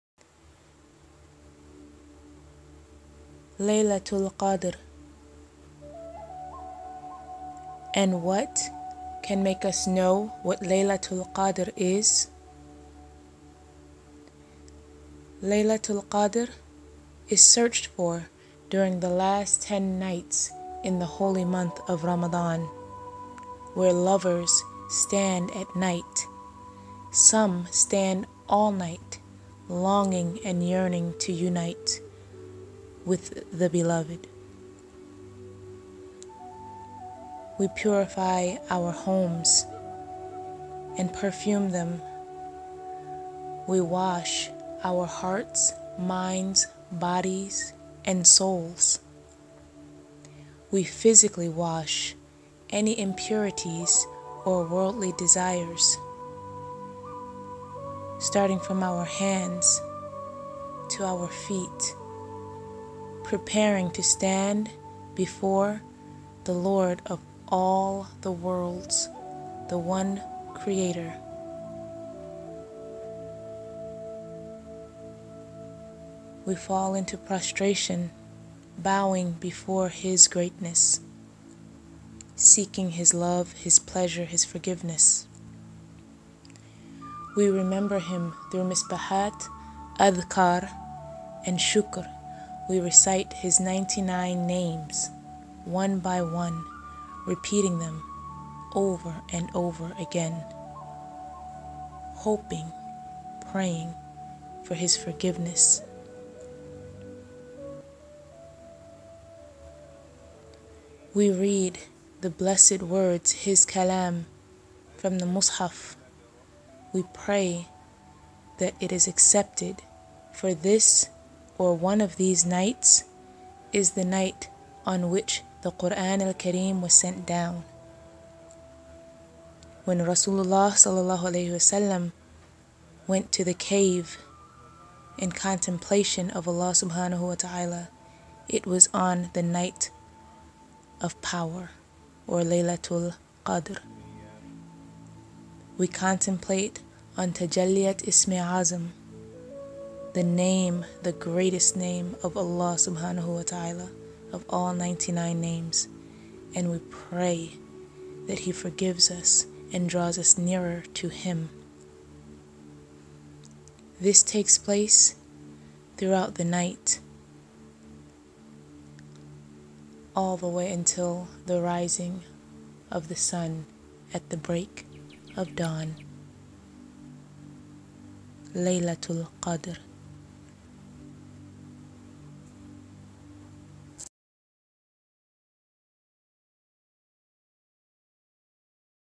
Lailatul Qadr? | The Night of Power? | A Muslim's Perspective | Better than 1,000 Nights
Peace be with you all.
This video was made in hopes to give people an idea of what Muslims spend their nights doing during the Night of Power. Because worship is so personal between the creation to the Creator, we decided it would be better to just B-role instead of filming our actual ibadah (worship). May Allah allow this to be of benefit to someone seeking the truth.
Ameen.
Things we did not emphasize on in this video is that Allah swt says that this night is better than 1,000 months and the Malaaikah (Angles) and Rüh (Spirits) descend on this night.